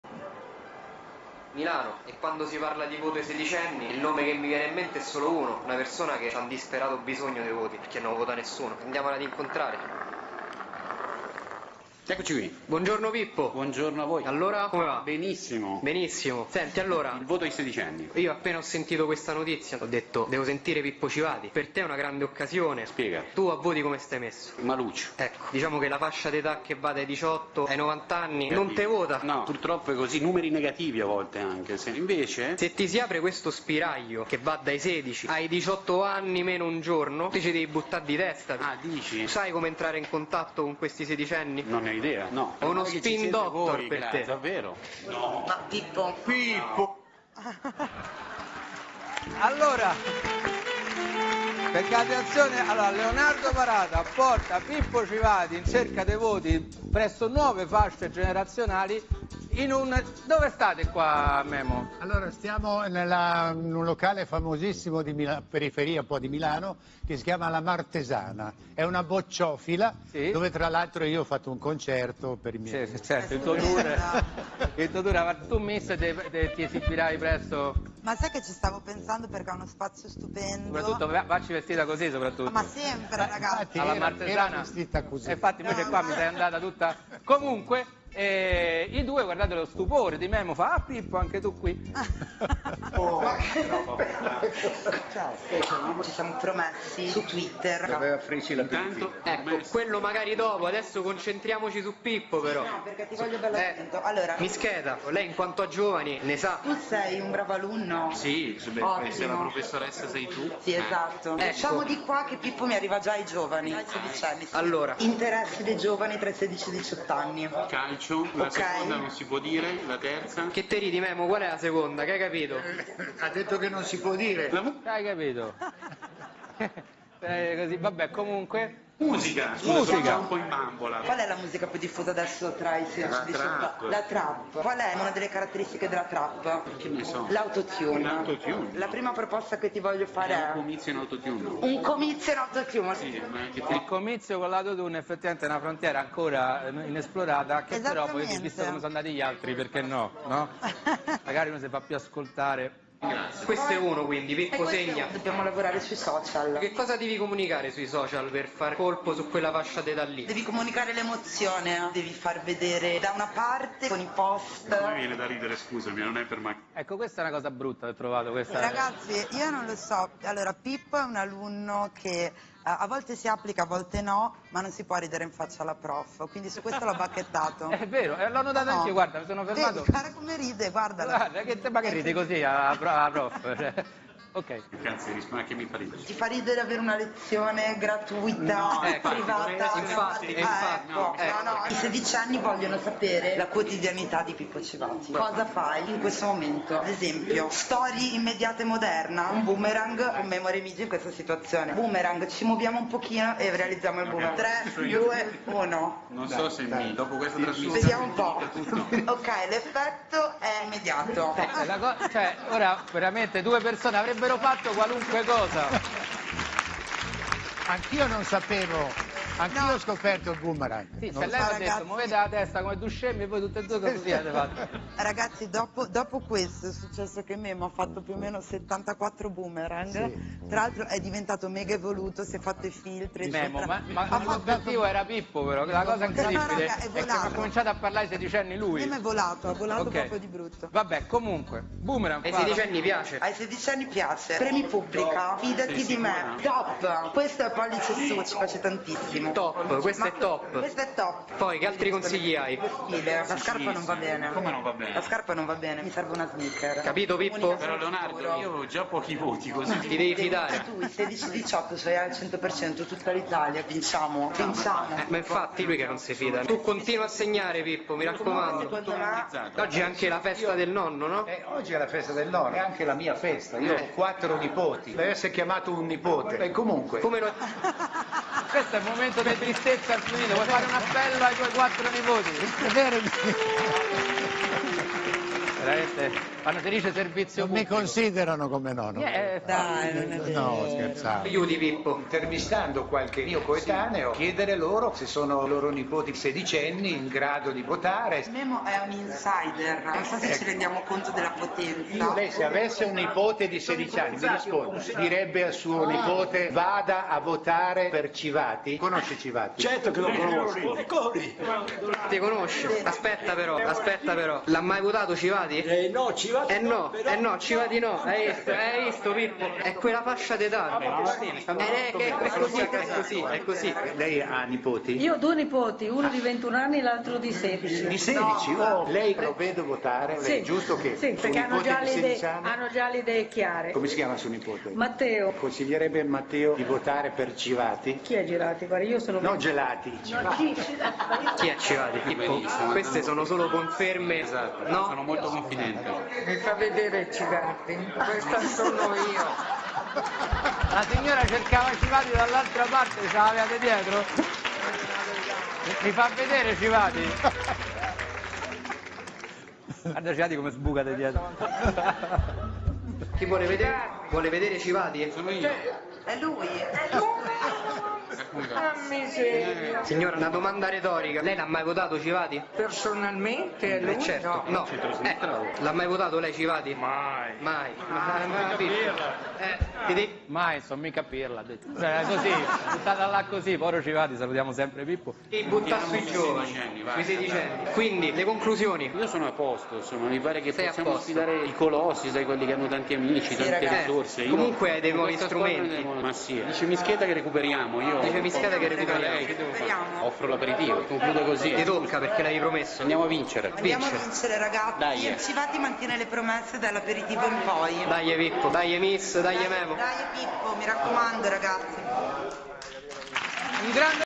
Voto a 16 anni, Pippo Civati a lezione di comunicazione da Myss Keta
Leonardo Parata e Memo Remigi a supporto della Prof. Myss Keta per una lezione speciale di comunicazione a Pippo Civati.